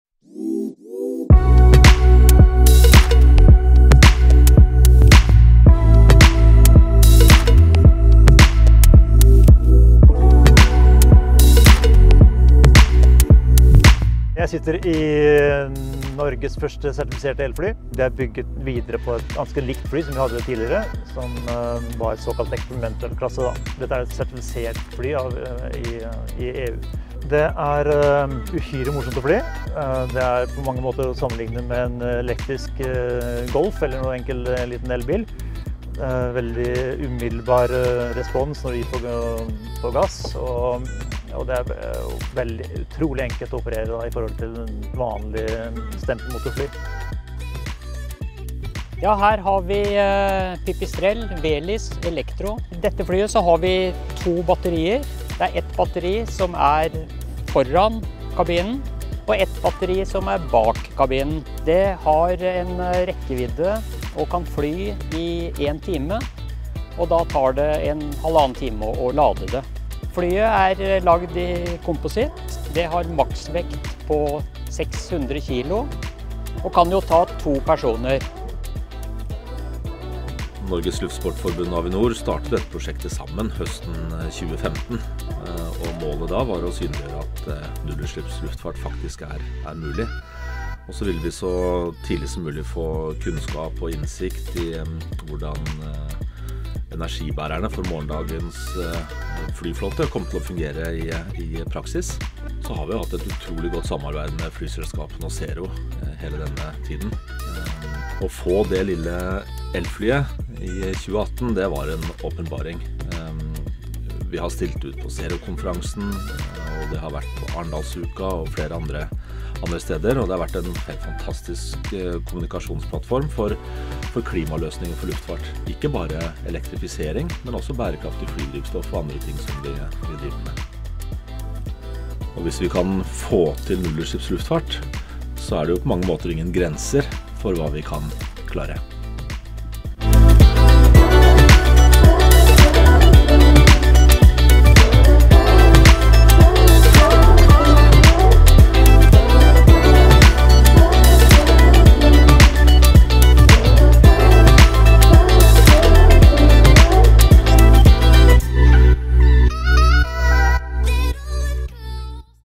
Avinor elfly
Dette er LN-ELB, Norges første EASA-sertifiserte elfly! Prosjektet er et samarbeid mellom Avinor, SAS, Widerøe, Norges Luftsportsforbund (NLF) og Zero. Mer informasjon: avinor.no/elfly